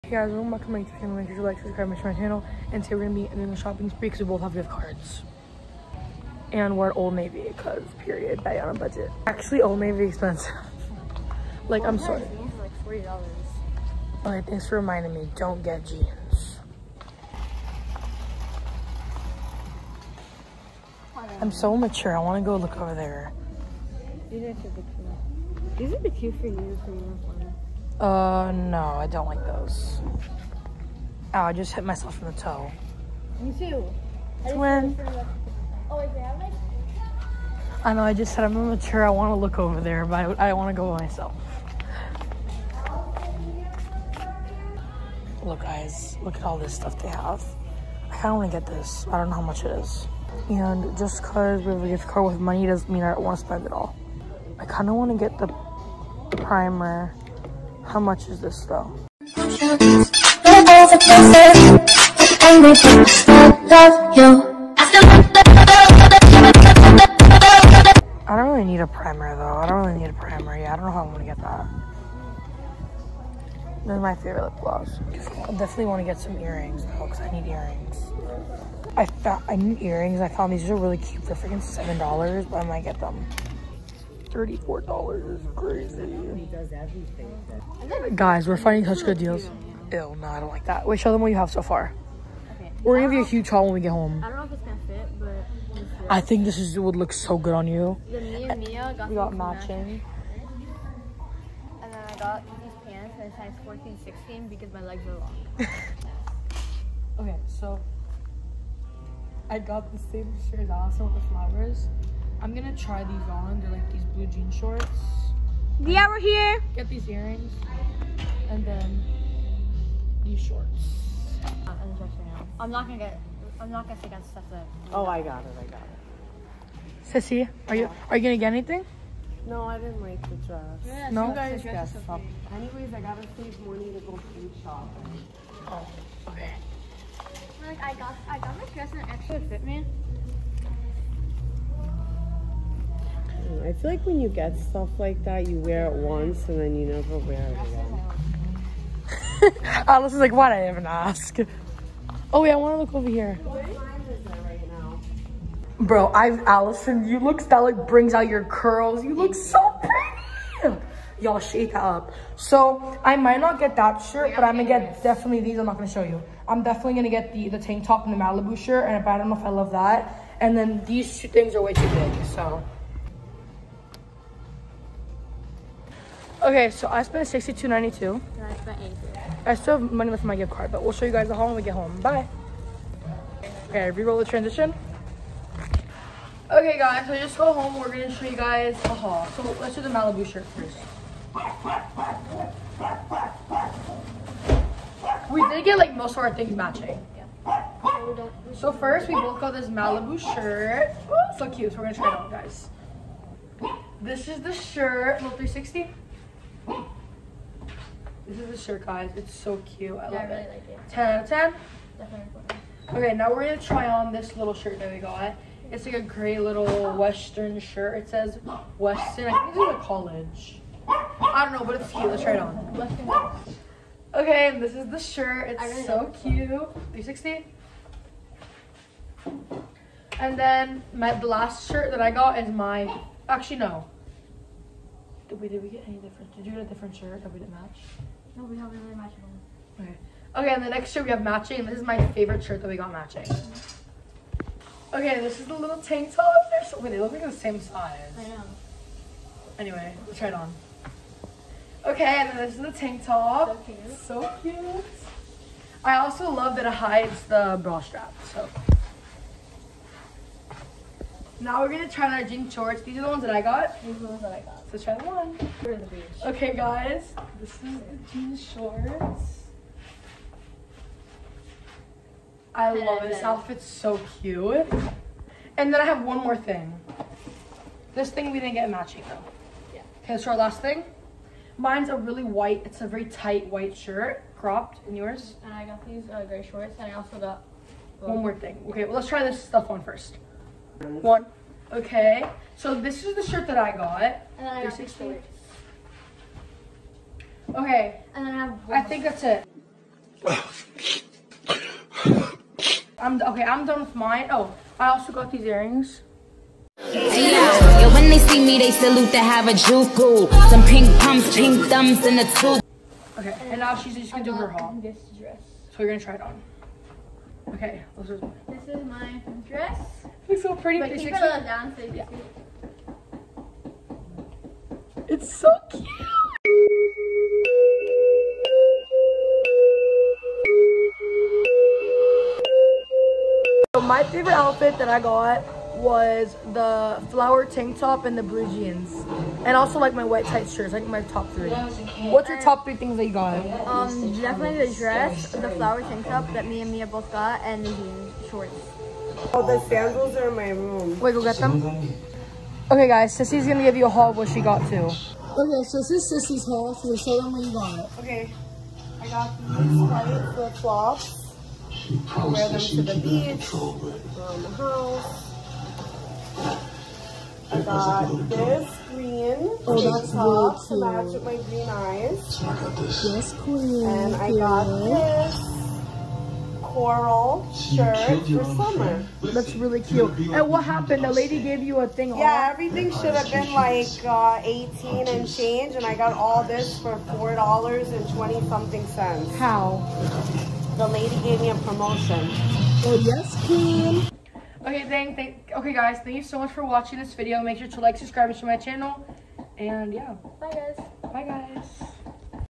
$100 Shopping Spree
**Title:** $100 Shopping Spree Challenge with My Cousin! 🛍️💸

**Description:**
Hey everyone! 🌟 Join me and my cousin as we take on the ultimate $100 shopping spree challenge! We'll be hitting up OLD NAVY, hunting for the best deals, and seeing who can get the most bang for their buck. From fashion finds to quirky gadgets, you won't believe what we snag for just $100 each. Watch till the end to see who comes out on top and to catch all our hilarious moments along the way.